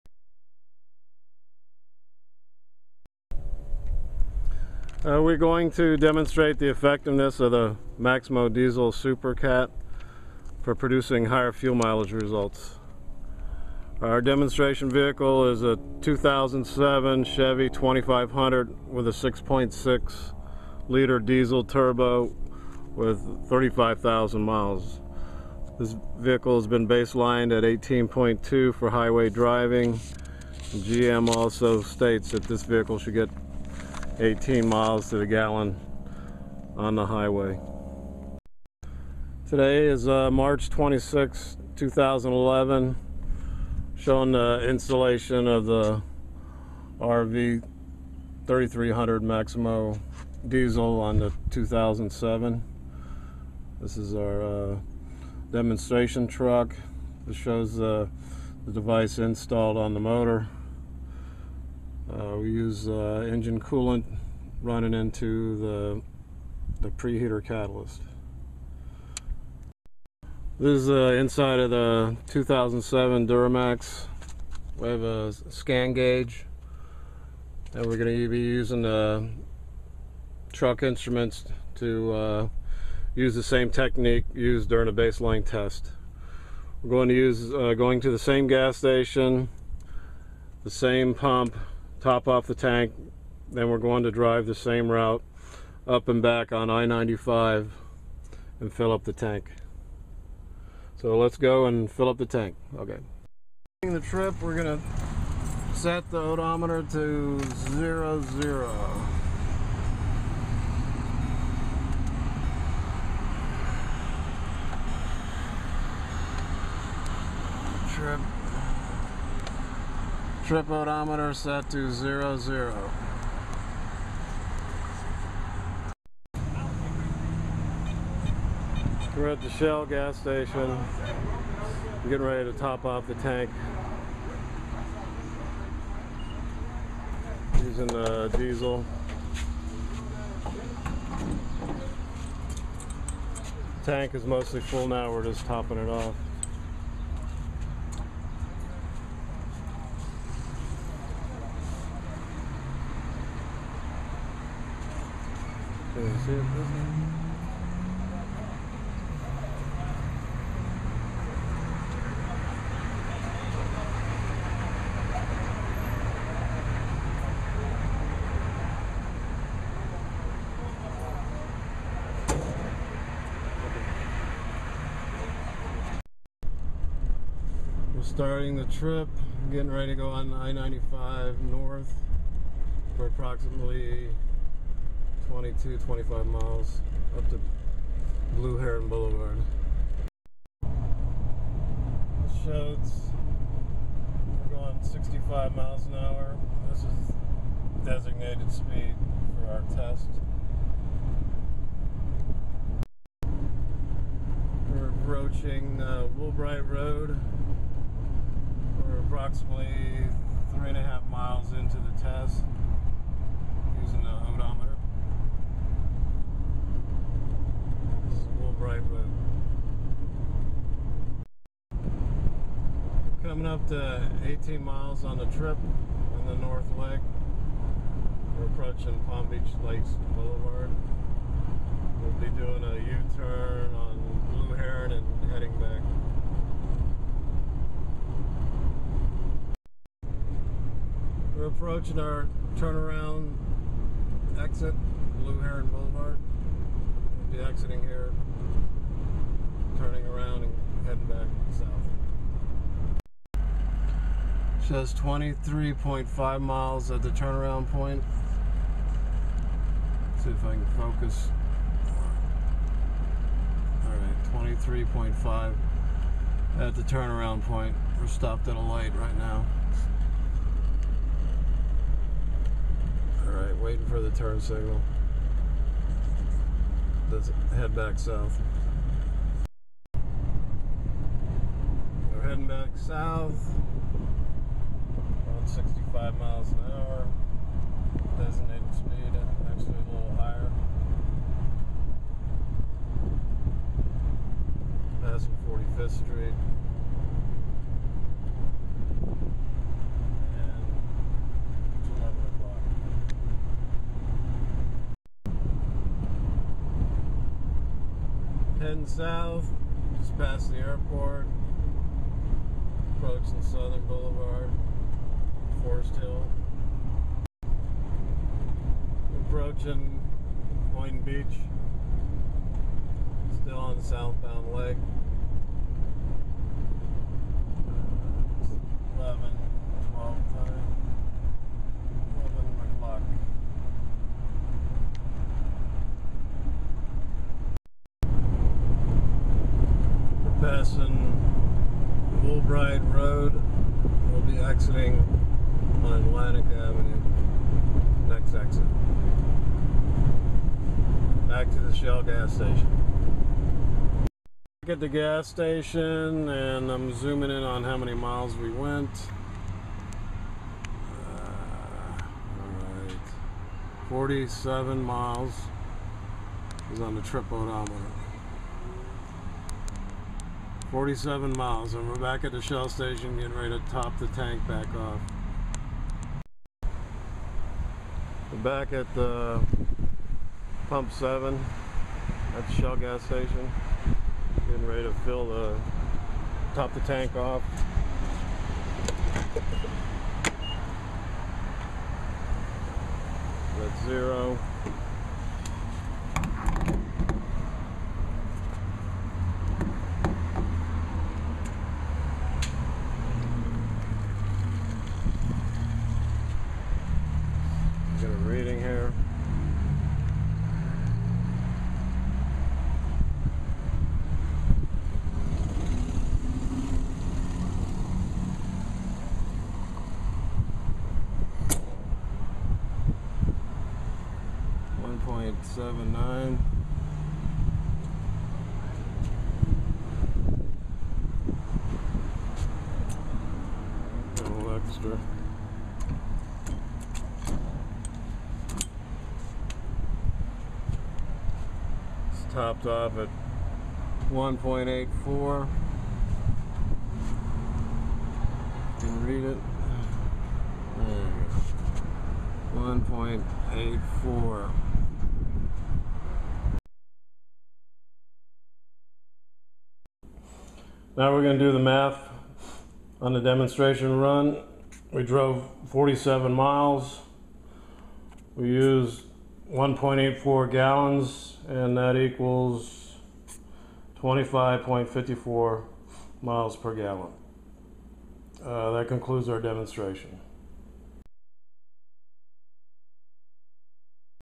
Fuel Saver Videos 2007 Duramax Diesel
Fuel Saver Video showing Maximo-Diesel fuel catalyst/pre-heater on 2007 Chevy Duramax 6.6 L Turbo Diesel.  Demo shows placement on engine and test protocol.  Tank is topped off and a 47 mile roundtrip circuit is driven.  Truck is baselined at 18.2 miles per gallon for highway driving.  Demo shows same drive circuit driven with pre-heater installed resulting in 25.5 miles per gallon.  For more info on the diesel products available